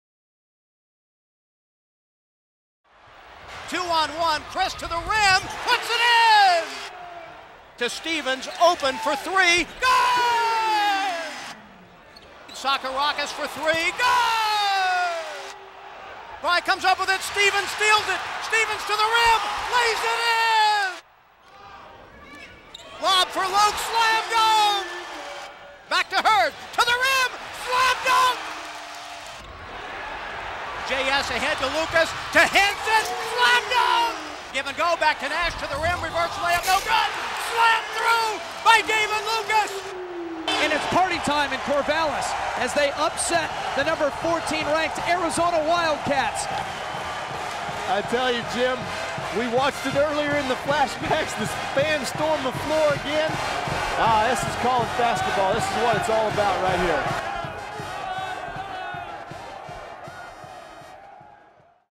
MBKB upsets #14 Arizona in 2004
Remember when David Lucas, Chris Stephens, J.S. Nash and Lamar Hurd, et al. upset #14 Arizona in 2004? Be in Gill to help the Beavers try for another upset of 3rd ranked Arizona this Wednesday, March 5th, at 8 PM. For tickets, go to beavertickets.com or call 1-800-GOBEAVS.